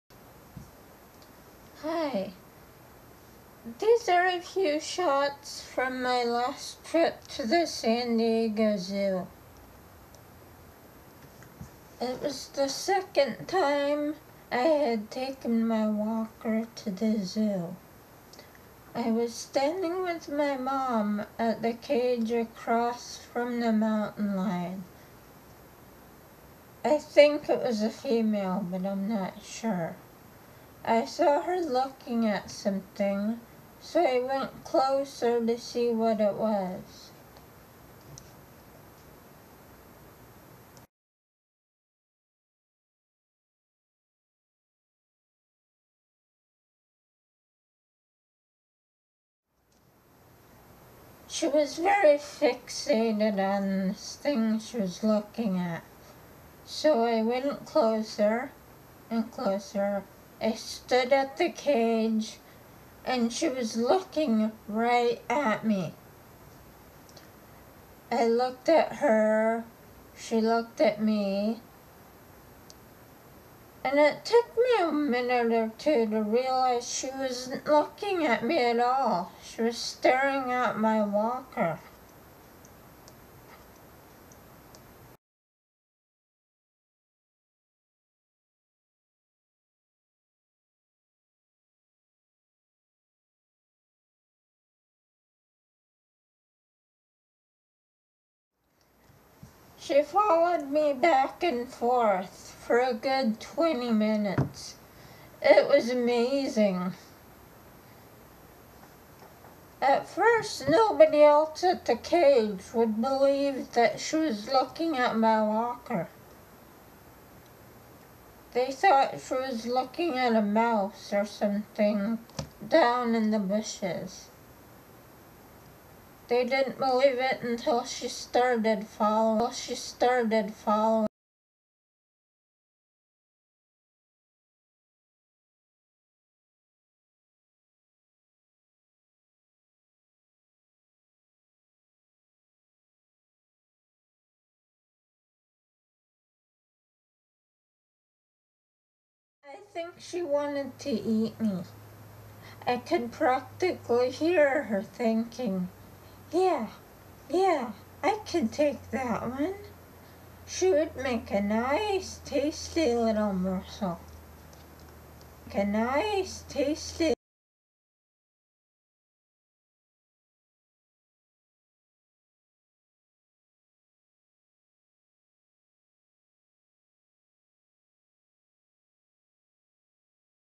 Mountain lion at the San Diego Zoo
Something caught this mountain lion's eye.